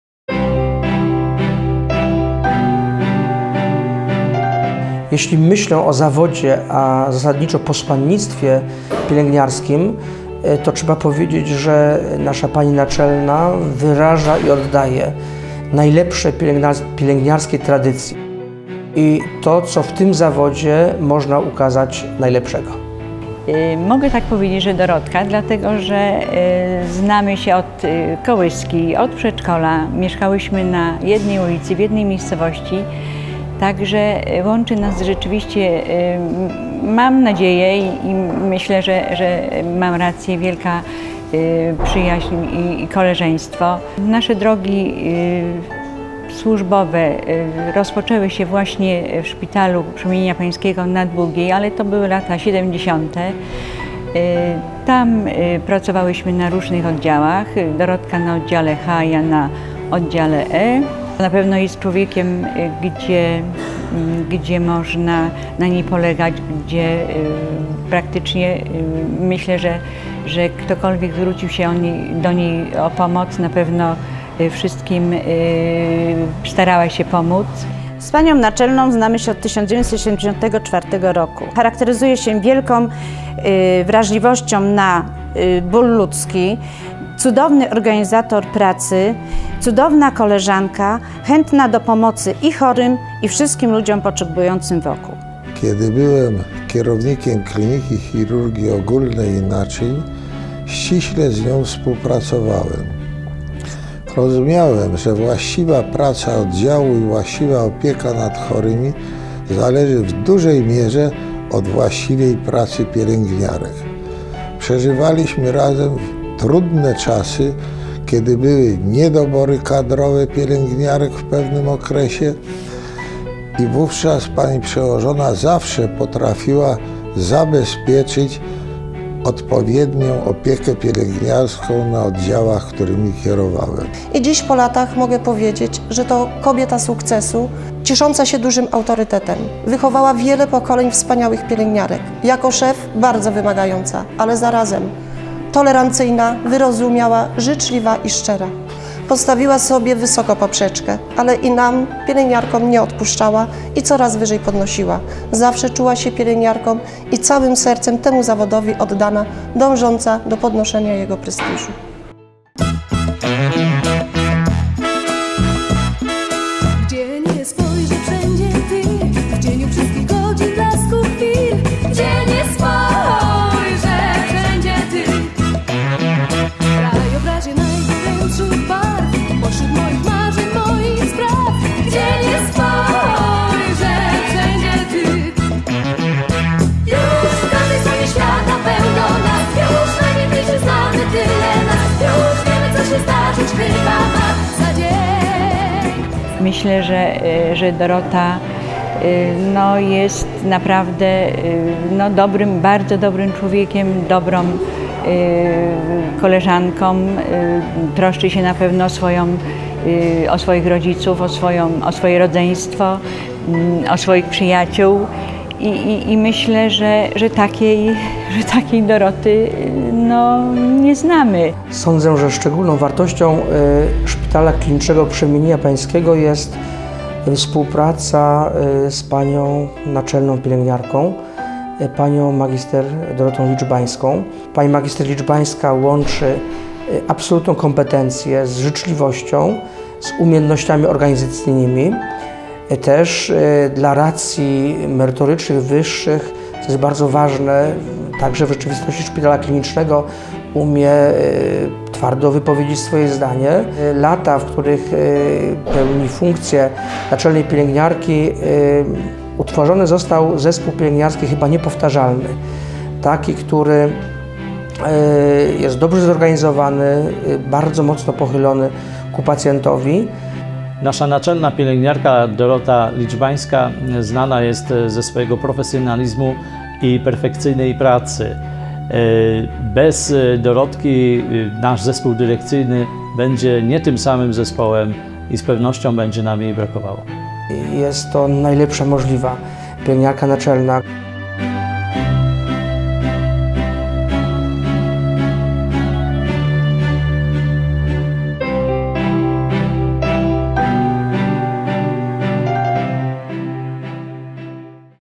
Dorota Liczbańska
Niezwykła postać. Dorota Liczbańska, wieloletnia Pielęgniarka Naczelna Szpitala Klinicznego Przemienienia Pańskiego UM w Poznaniu, dziś już na emeryturze. Zapraszamy na krótką chwilę wspomnień.